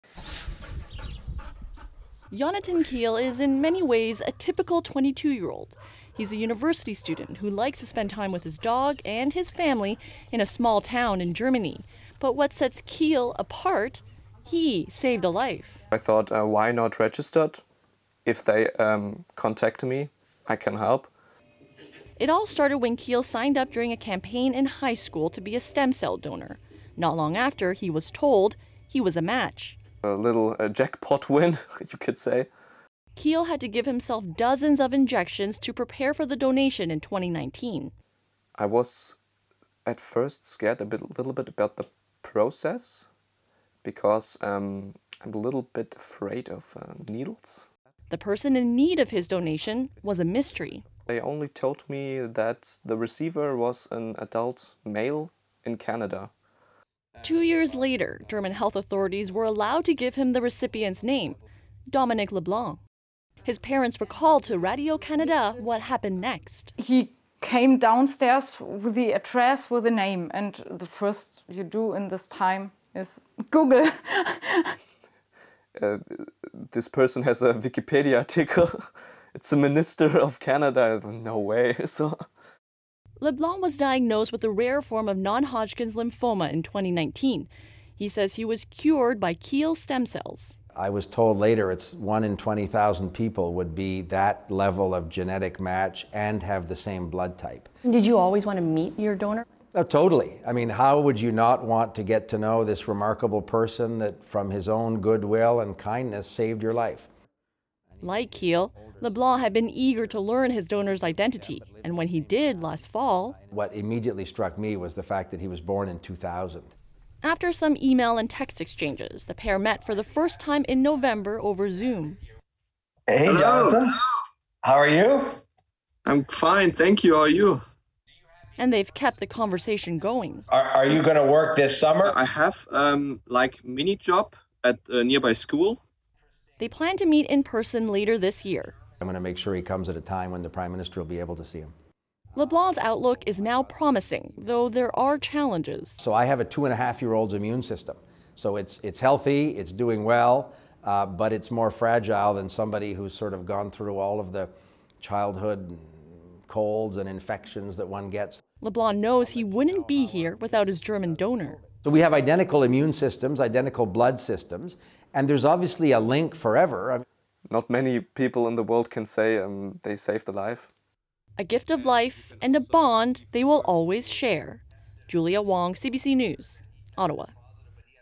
German stem cell donor saves Canadian cabinet minister's life
An anonymous stem cell donation from a young German man, helped saved Intergovernmental Affairs Minister Dominic LeBlanc's life. The two finally met virtually and LeBlanc looks forward to meeting Jonathan Kehl in person.